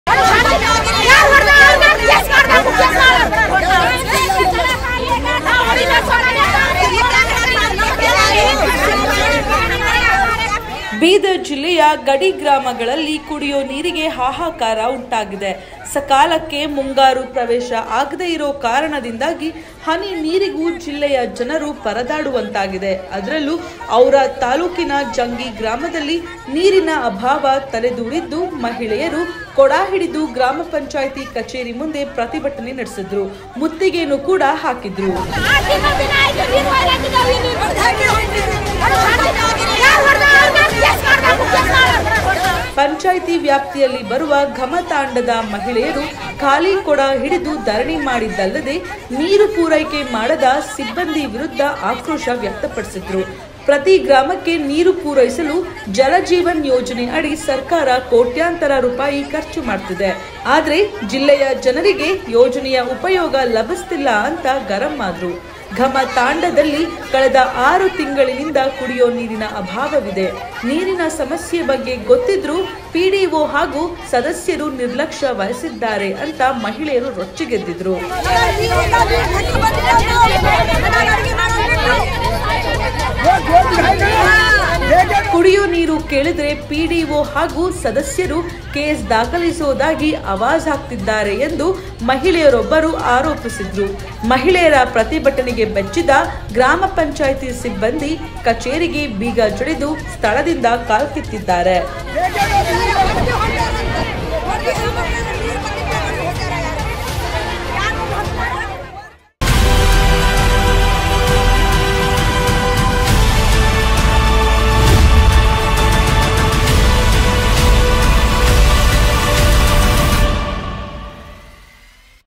No Water, No Peace | Women Protest for Water in bidar | Live News kannada
ಕುಡಿಯೋ ನೀರು ಪೂರೈಸುವಂತೆ ಒತ್ತಾಯಿಸಿ ಬೀದರ್ ಜಿಲ್ಲೆ ಔರಾದ್ ತಾಲೂಕಿನ ಜಂಗಿ (ಬಿ) ಗ್ರಾಮದಲ್ಲಿ ಮಹಿಳೆಯರು ಧರಣಿ ನಡೆಸಿದ್ದಾರೆ.

ನೀರು ಪೂರೈಸದಿದ್ರೆ ಮಹಿಳೆಯರ ಸಿಟ್ಟು ಯಾವ್ ಪರಿ ನೆತ್ತಿಗೆ ಏರುತ್ತೆ ಅನ್ನೋದಕ್ಕೆ ಈ ಸುದ್ದಿನೇ ಸಾಕ್ಷಿ.. ಕಳೆದ ೬ ತಿಂಗಳಿAದ ನೀರಿನ ಬವಣೆ
ಅನುಭವಿಸಿದ ಮಹಿಳೆಯರು ಖಾಲಿ ಕೊಡ ಹಿಡಿದು ಸೀದಾ ಗ್ರಾಮ ಪಂಚಾಯ್ತಿ ಕಡೆ ಬಂದಿದ್ರು. ನೀರು ಪೂರೈಕೆ ಮಾಡೋ ತನಕ ನಾವ್ ಕದಲಲ್ಲ
ಅಂತಾ ಪಟ್ಟು ಹಿಡಿದಿದ್ರು.